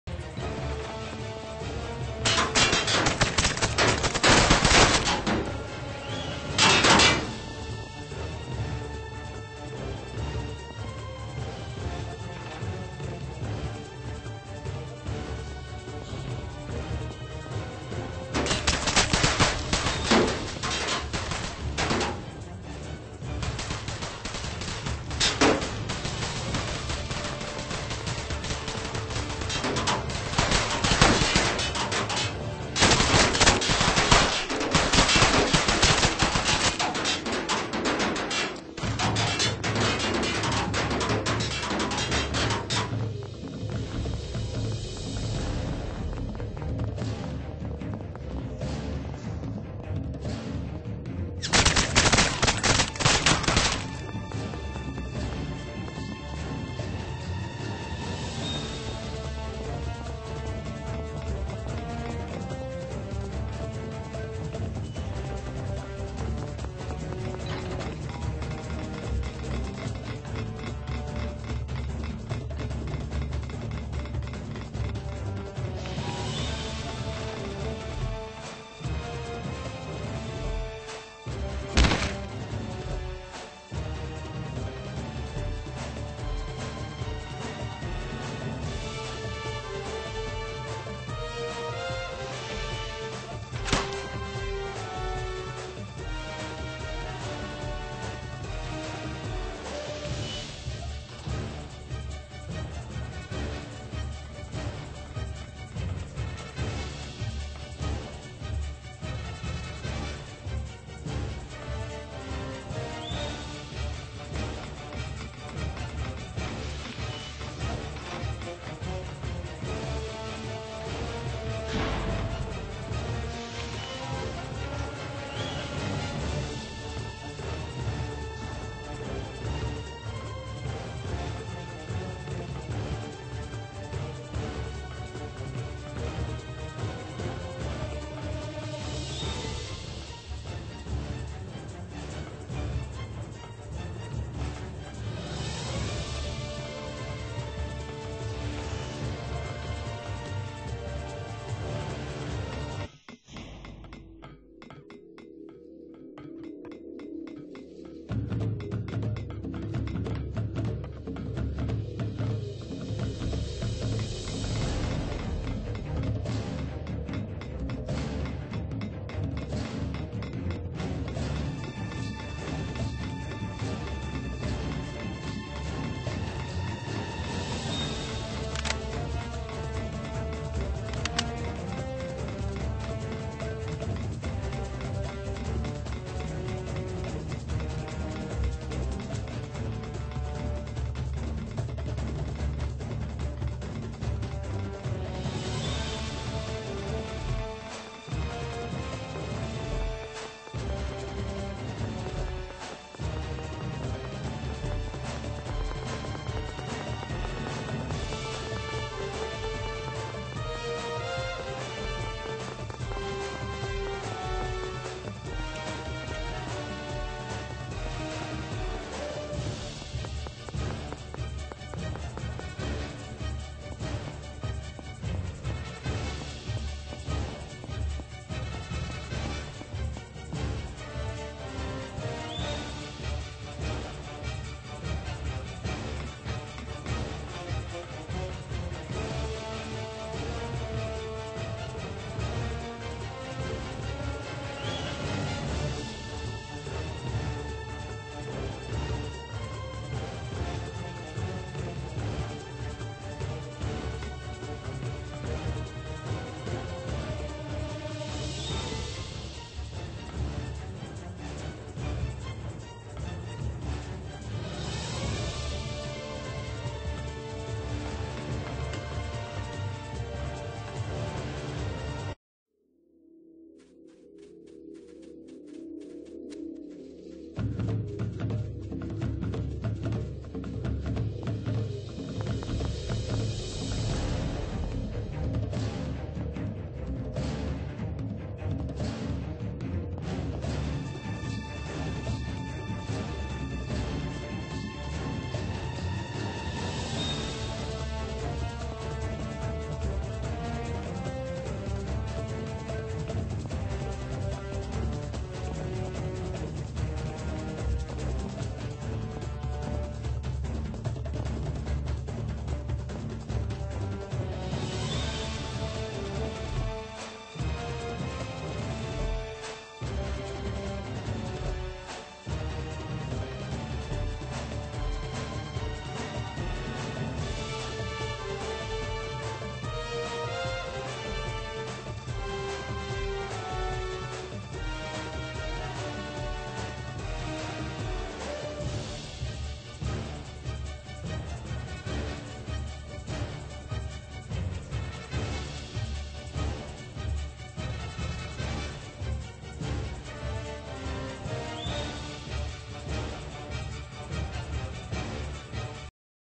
UIU BREACHING THE CDC AFTER A VIRUS INFECTS ALL THE CLASS-D
UIU BREACHING CDC AND KILLING ALL THE INFECTED.